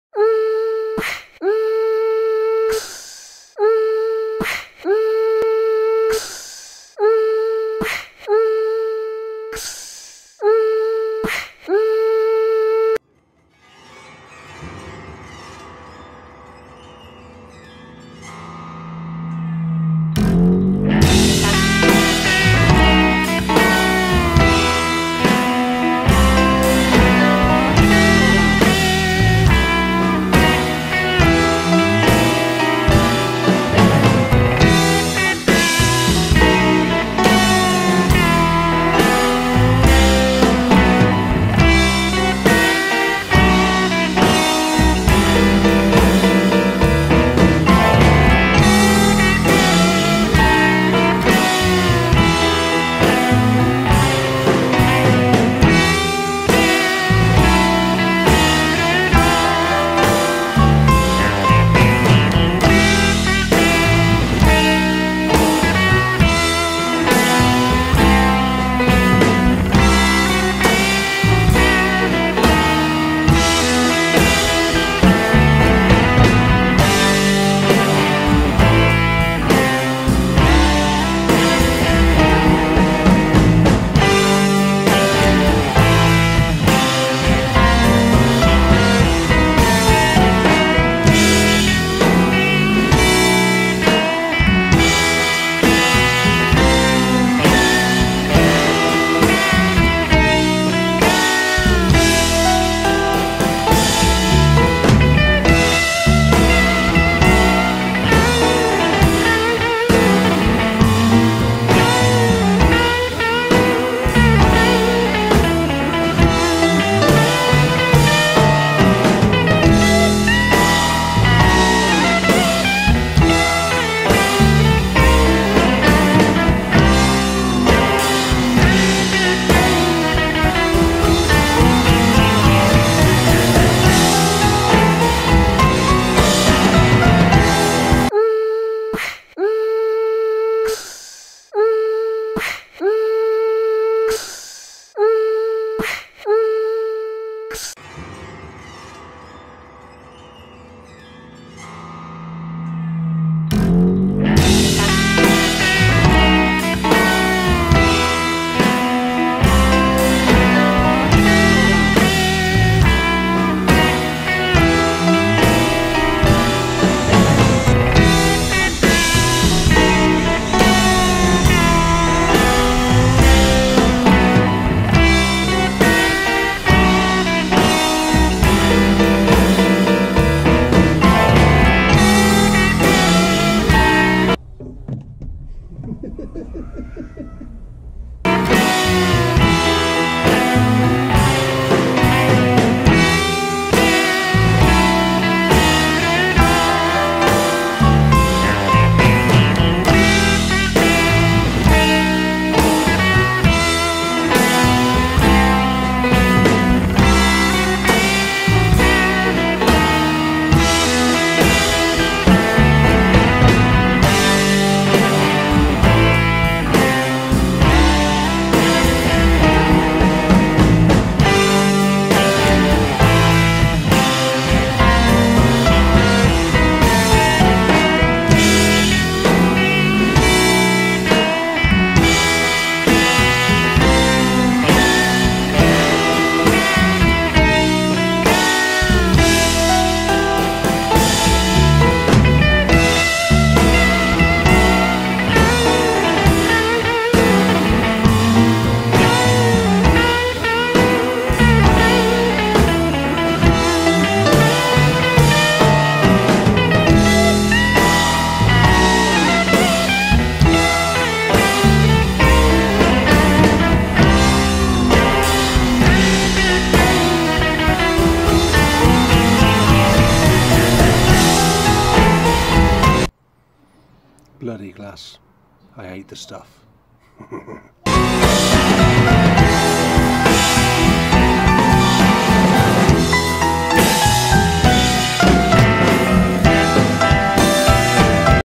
Making A Glass Top For The 39.6 Kilo (87.4lbs) Branch Garden Table Lump
In my last video I made a heavy garden table out of branches. In this video I made a template for a custom made glass top and fitted it, finishing it off and giving it a edge and contrast.